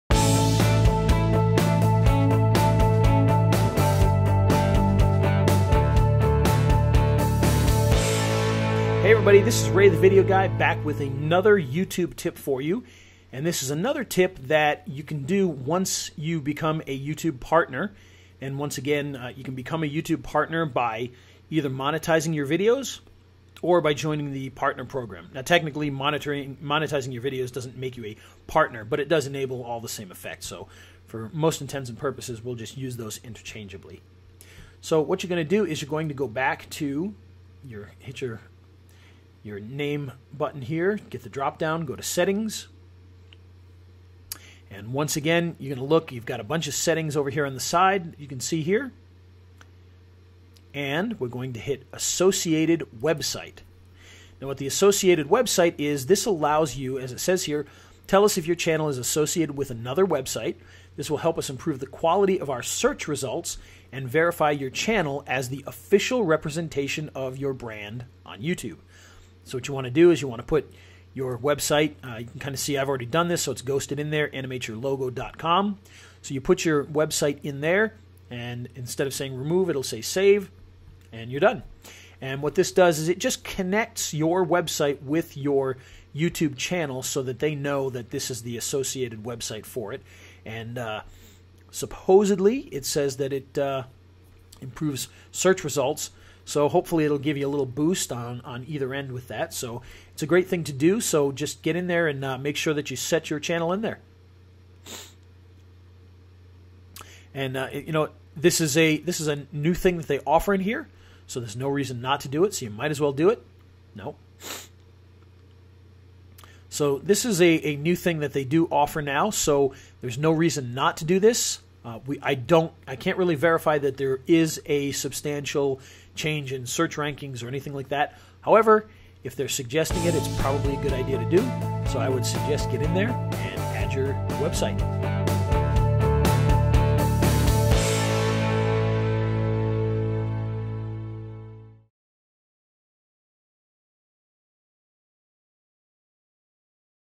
Youtube Tips and Tricks Setting an Associated Website for your channel - Ray The Video Guy
For those

If you are using YouTube for your business, or brand, you can associate a site with your YouTube channel.  All you have to do is visit your YouTube settings, go to "Associated Website" and enter your site into the box.

You will then be required to validate the site by adding a file onto your website's server.

YouTube Tips and Tricks